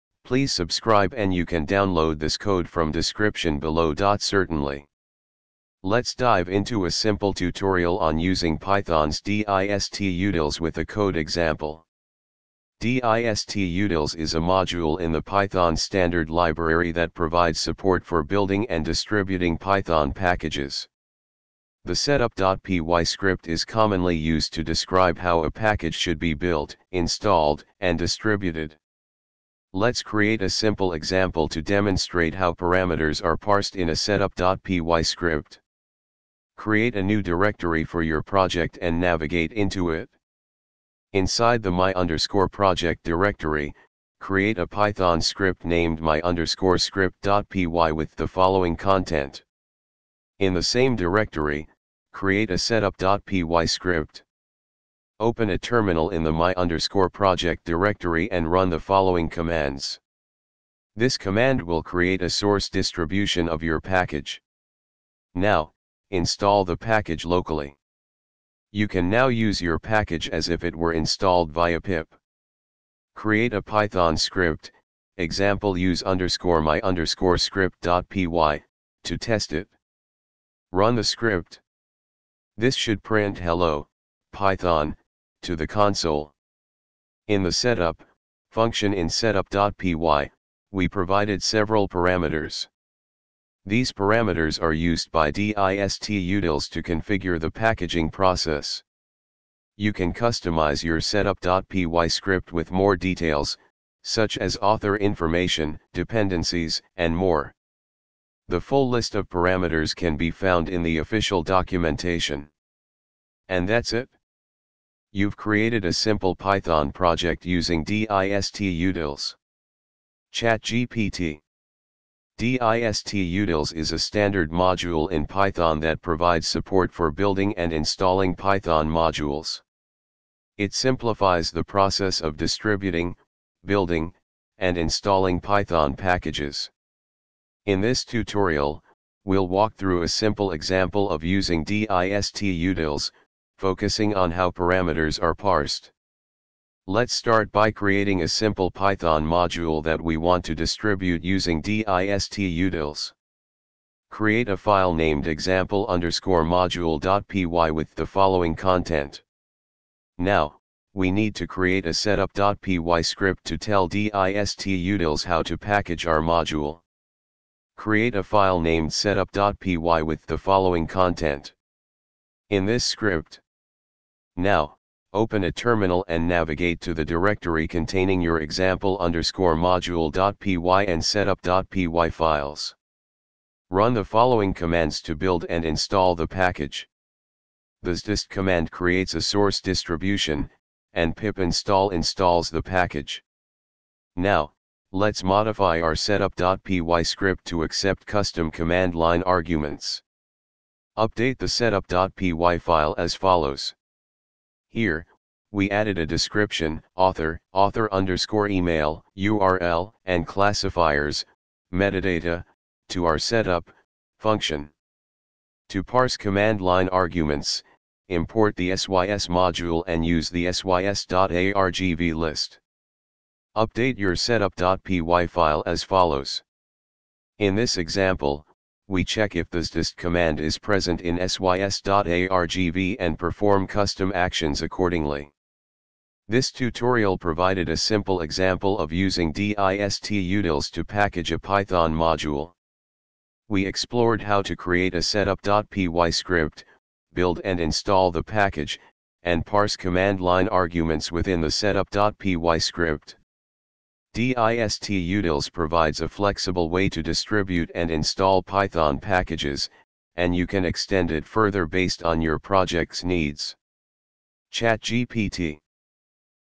python distutils simple example how are parameters parsed
Certainly! Let's dive into a simple tutorial on using Python's distutils with a code example. distutils is a module in the Python Standard Library that provides support for building and distributing Python packages. The setup.py script is commonly used to describe how a package should be built, installed, and distributed.
Let's create a simple example to demonstrate how parameters are parsed in a setup.py script.
Create a new directory for your project and navigate into it:
Inside the my_project directory, create a Python script named my_script.py with the following content:
In the same directory, create a setup.py script:
Open a terminal in the my_project directory and run the following commands:
This command will create a source distribution of your package.
Now, install the package locally:
You can now use your package as if it were installed via pip. Create a Python script (e.g., use_my_script.py) to test it:
Run the script:
This should print "Hello, Python!" to the console.
In the setup() function in setup.py, we provided several parameters:
These parameters are used by distutils to configure the packaging process.
You can customize your setup.py script with more details, such as author information, dependencies, and more. The full list of parameters can be found in the official documentation.
And that's it! You've created a simple Python project using distutils.
ChatGPT
Distutils is a standard module in Python that provides support for building and installing Python modules. It simplifies the process of distributing, building, and installing Python packages. In this tutorial, we'll walk through a simple example of using Distutils, focusing on how parameters are parsed.
Let's start by creating a simple Python module that we want to distribute using Distutils. Create a file named example_module.py with the following content:
Now, we need to create a setup.py script to tell Distutils how to package our module. Create a file named setup.py with the following content:
In this script:
Now, open a terminal and navigate to the directory containing your example_module.py and setup.py files. Run the following commands to build and install the package:
The sdist command creates a source distribution, and pip install installs the package.
Now, let's modify our setup.py script to accept custom command-line arguments. Update the setup.py file as follows:
Here, we added a description, author, author_email, url, and classifiers (metadata) to our setup() function.
To